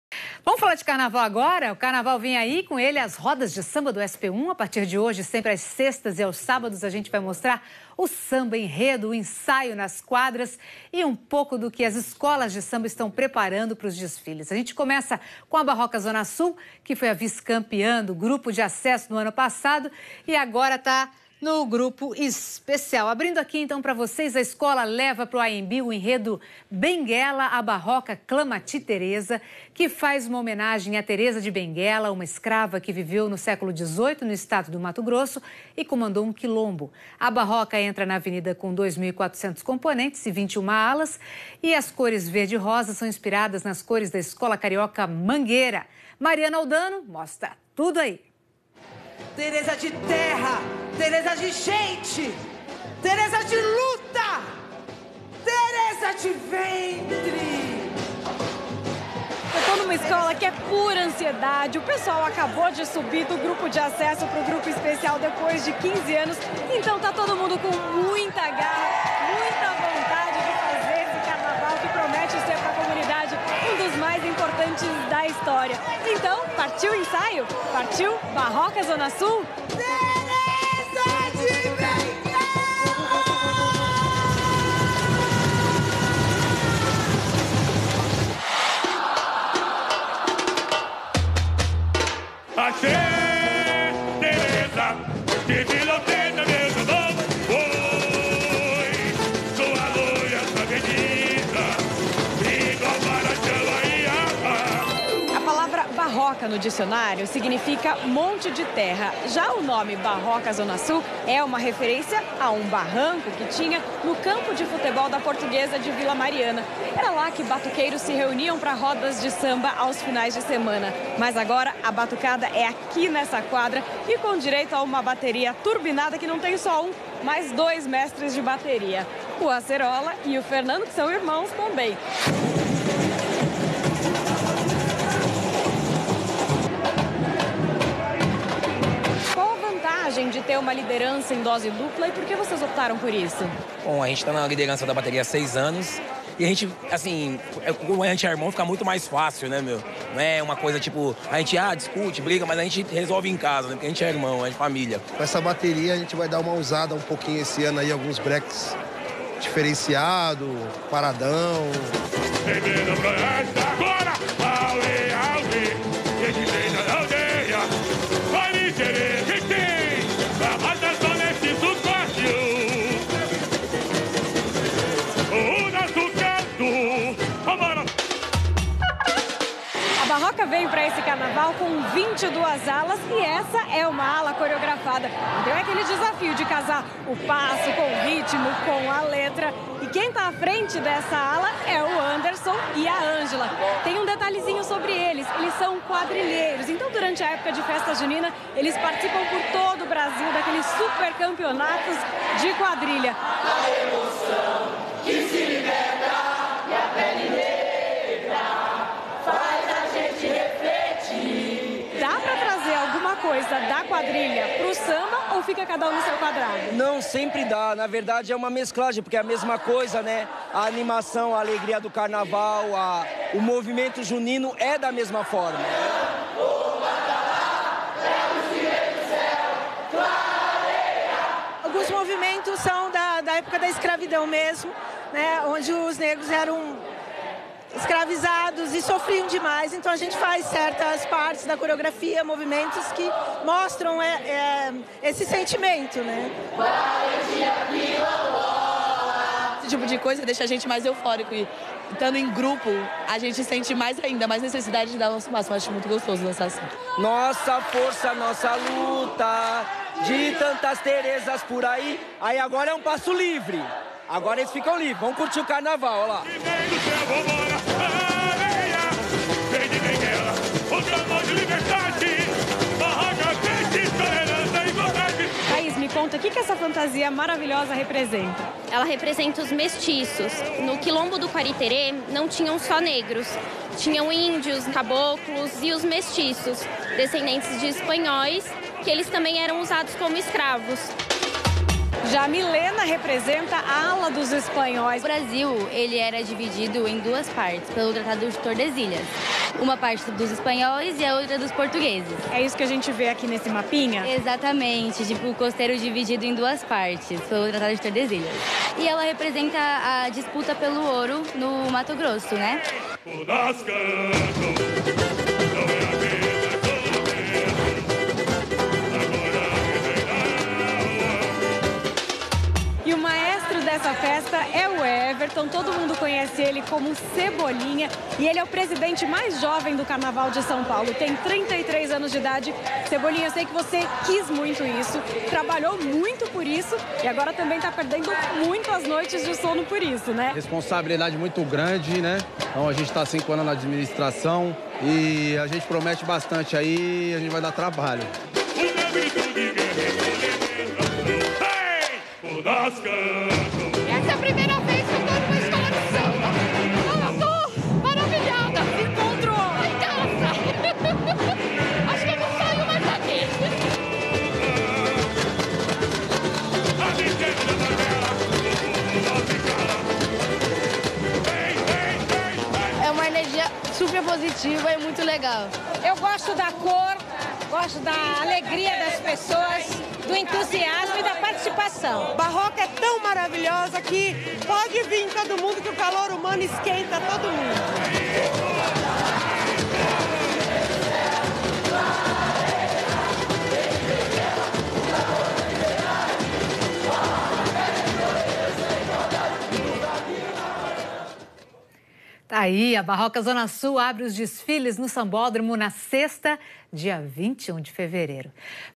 SP 1 - RODA DE SAMBA 2020 - BARROCA ZONA SUL
RODAS DE SAMBA DO SP1 APRESENTAM SAMBAS-ENREDO É DETALHES DAS ESCOLAS PARA O CARNAVAL 2020

A BARROCA ZONA SUL ABRE OS DESFILES DO SAMBÓDROMO NA SEXTA FEIRA, 21 DE FEVEREIRO.